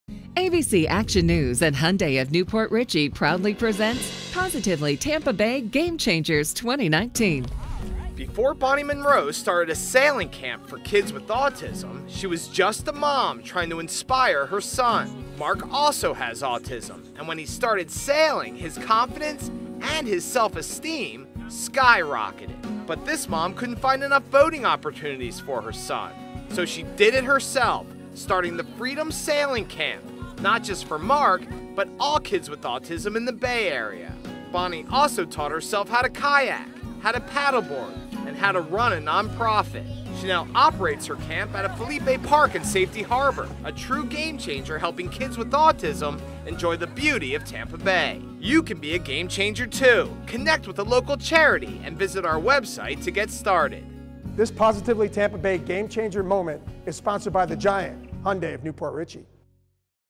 Bonnie Monroe - September's Game Changer
One local mom started the Freedom Sailing Camp for kids with autism.